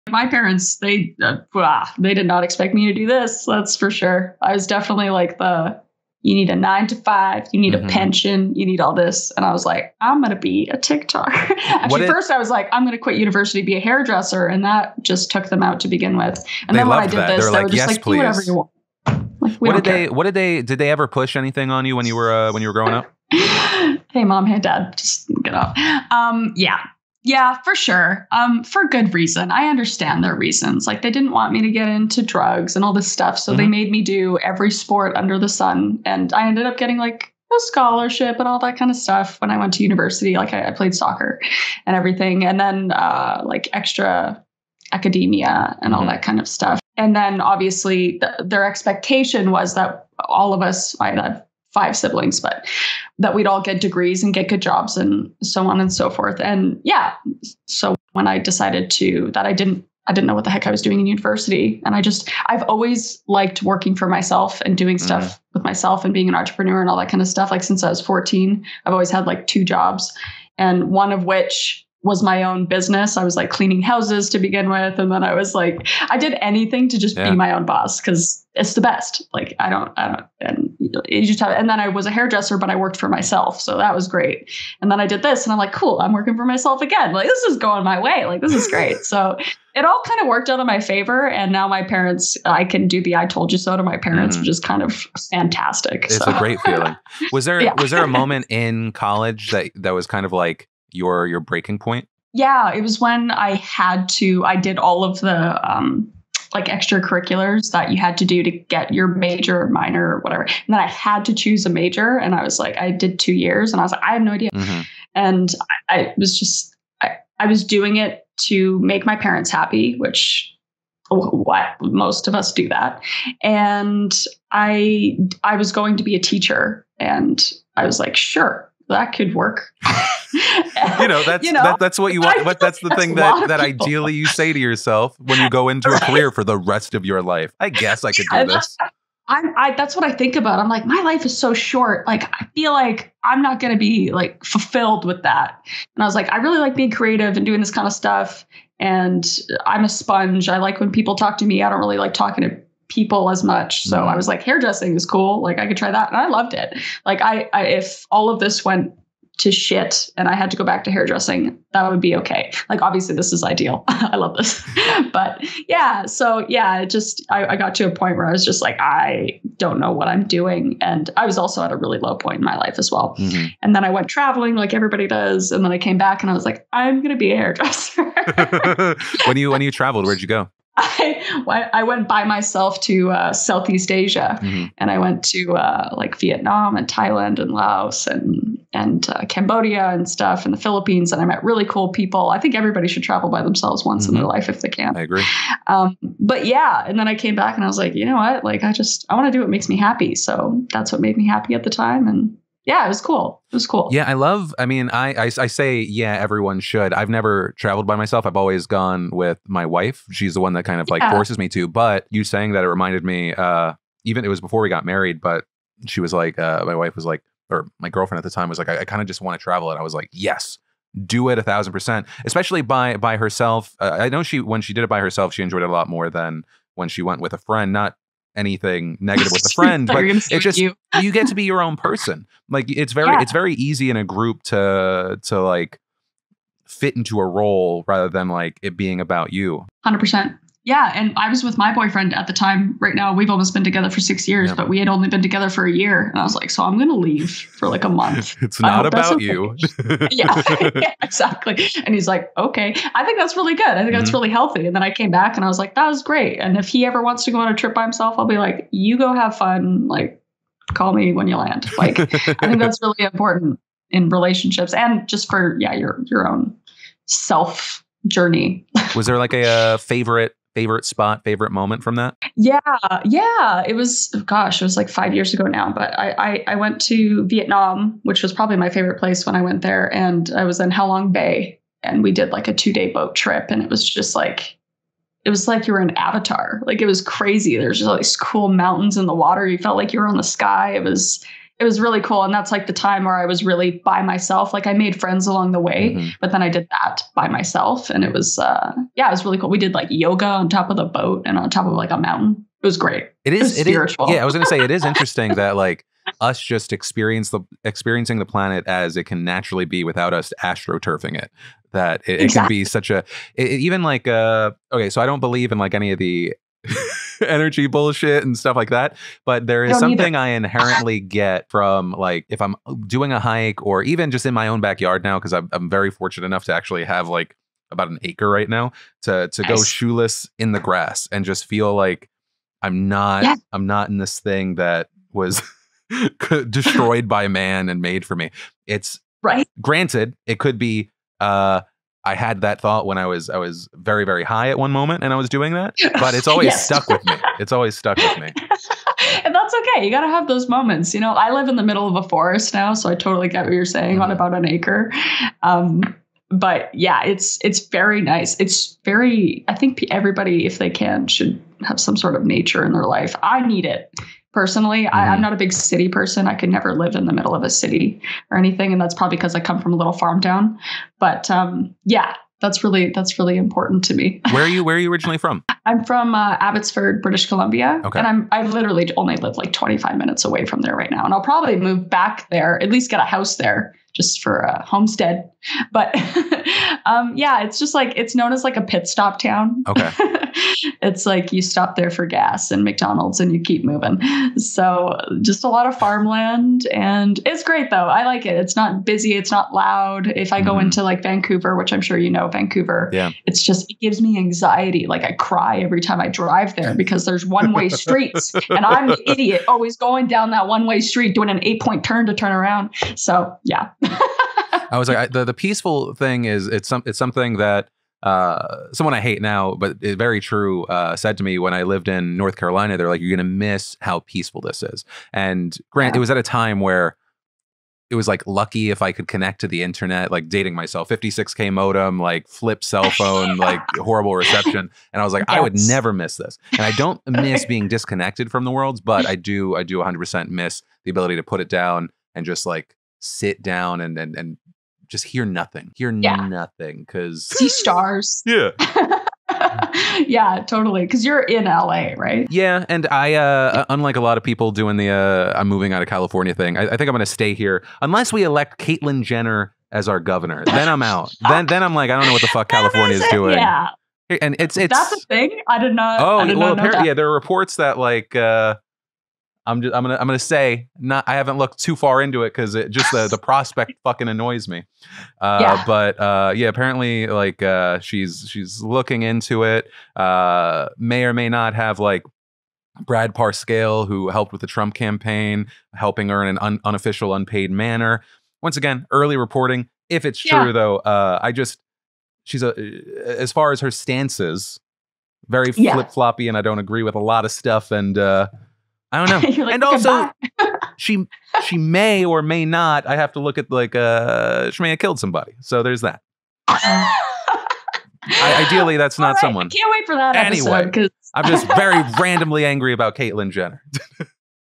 KallMeKris On Why She Won't Ever Have A Boss Again, Traveling Alone, and Finding HAPPINESS!
for limited edition merch! Ends soon.
Edited by:
William Crespo